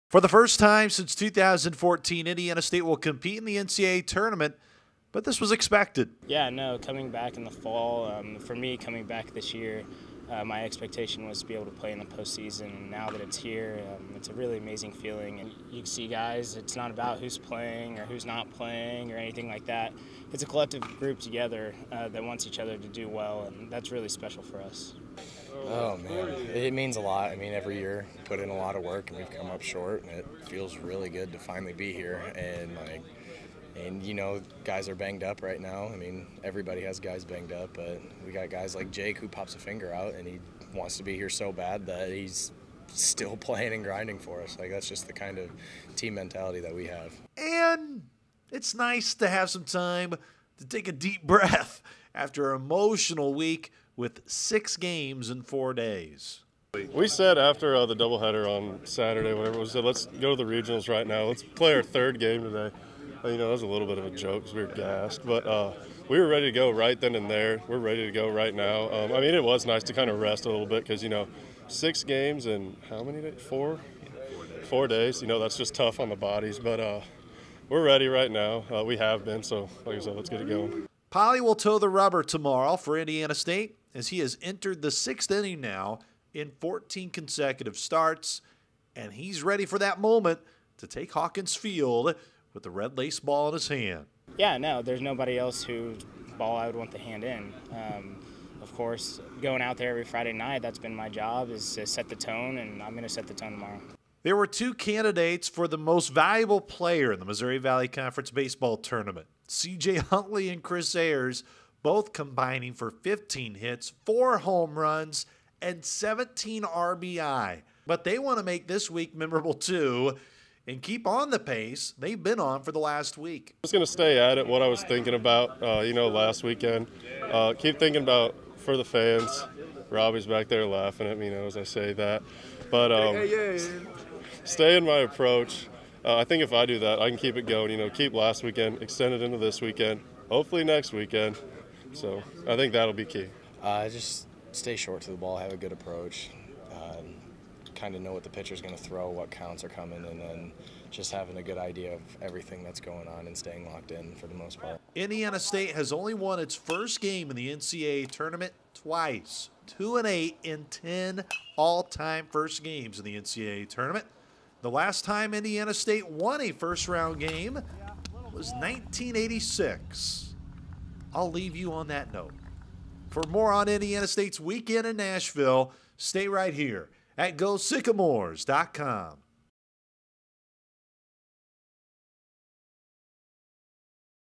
BASE: Sycamores Ready For NCAA Stage
Indiana State opens NCAA Regional play  tomorrow against McNeese.